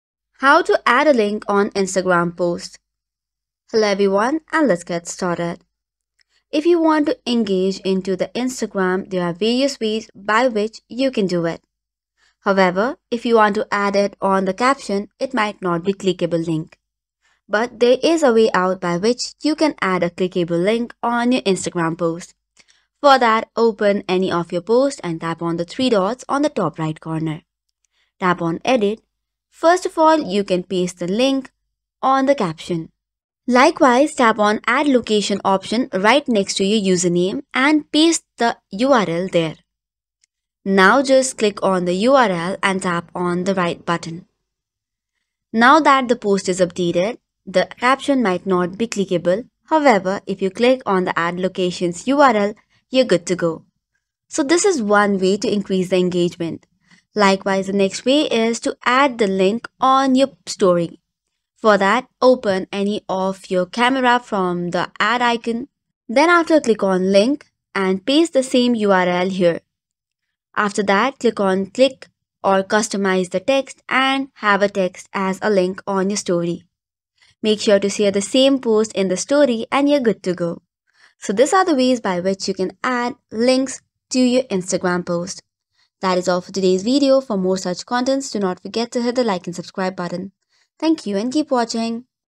How to Add Link To Instagram Post (Step by Step)
Want to add a clickable link to your Instagram post? Learn how to do it easily with this step-by-step guide. Drive traffic to your website, blog, or other online content by including a link directly in your Instagram posts.

In this tutorial:
0:00 - Introduction
0:05 - How to Add Link To Instagram Post

Discover the simple process of adding a clickable link to your Instagram post. This comprehensive tutorial ensures you can include links seamlessly, allowing you to promote your content and drive engagement effectively.

Enhance your Instagram posts with clickable links by following this guide!